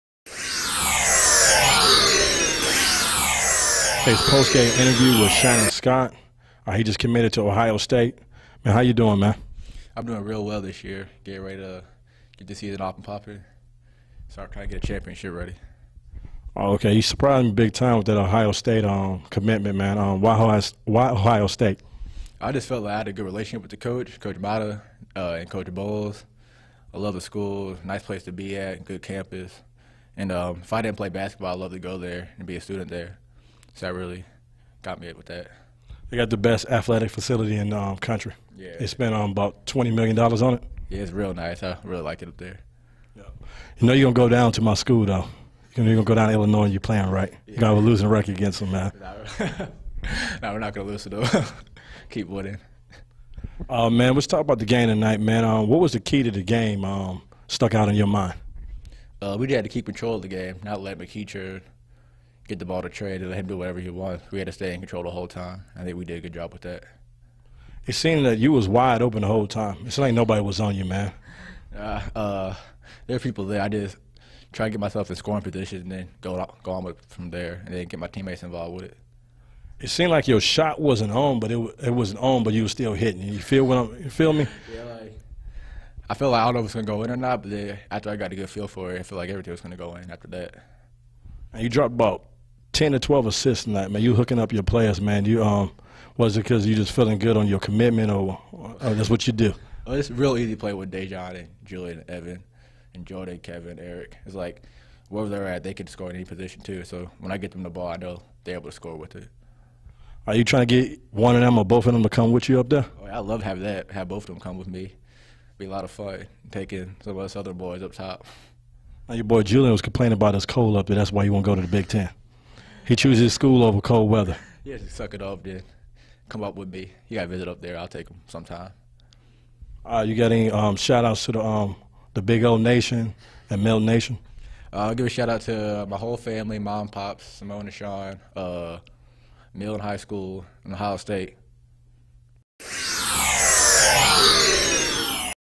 Post Game Interview Shannon Scott
What is there to say about Ohio States future point guard Shannon Scott? You could turn off every light in the gym and with his cat-like vision, he would still find his teammates open. Knocking down open jumpers or with a hand in his face, he was a man among boys.